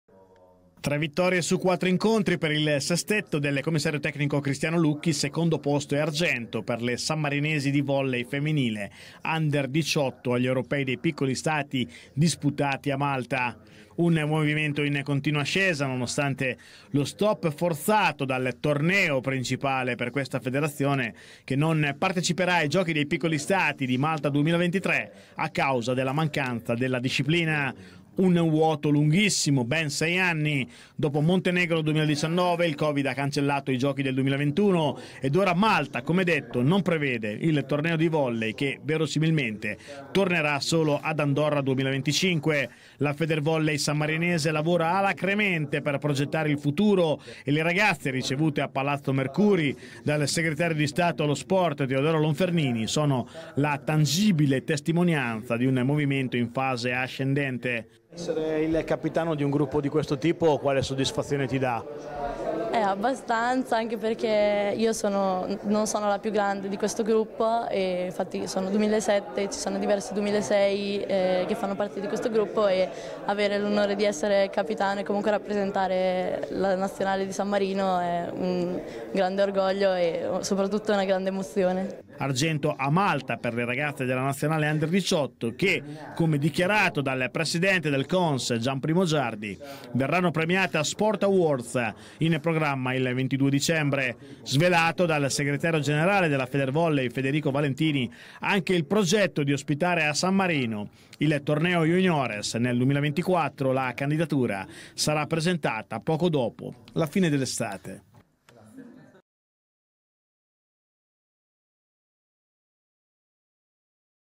I complimenti di Lonfernini alle ragazze d'argento del Volley
Il Segretario di Stato allo Sport riceve e si complimenta con le ragazze Argento agli Europei dei Piccoli Stati